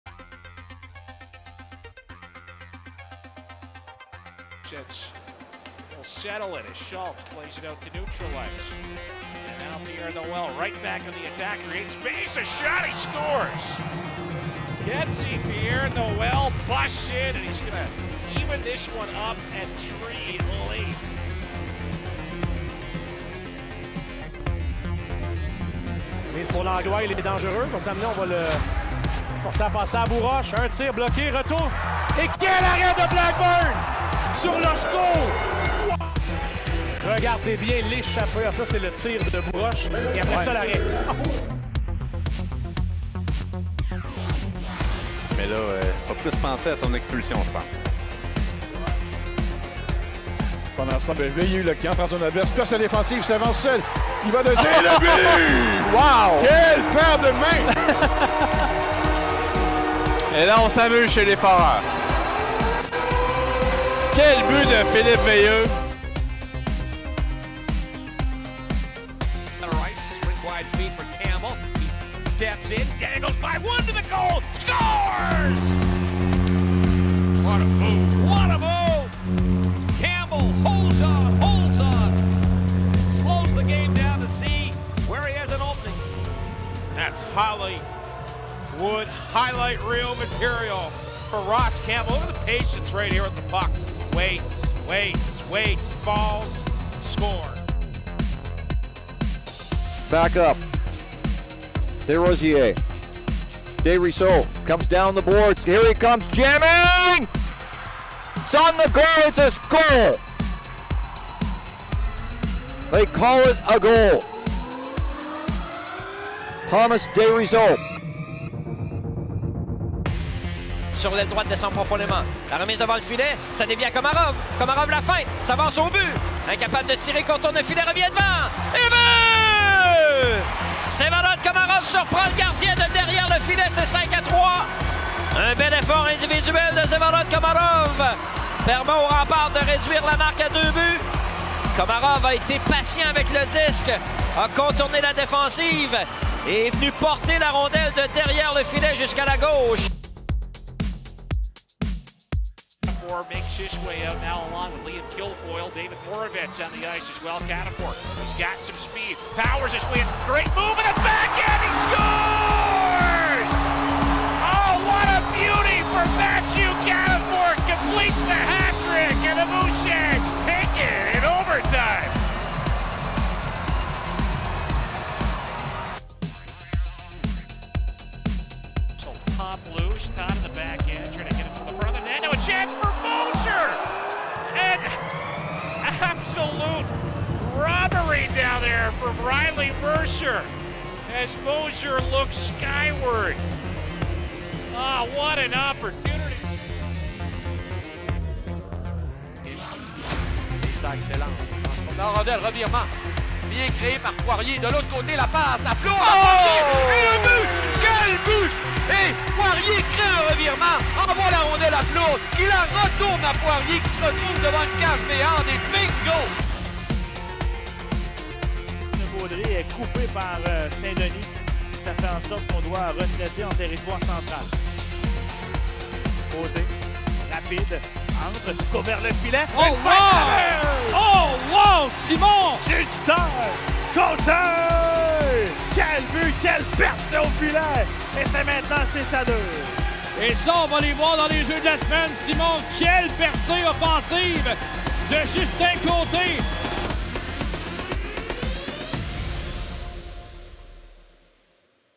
Top 10 - Jeux du mois LHJMQ - QMJHL Plays of the Month – Dec. 2023
Les plus beaux jeux de la LHJMQ durant le mois de décembre 2023. The top QMJHL plays from the month of December 2023.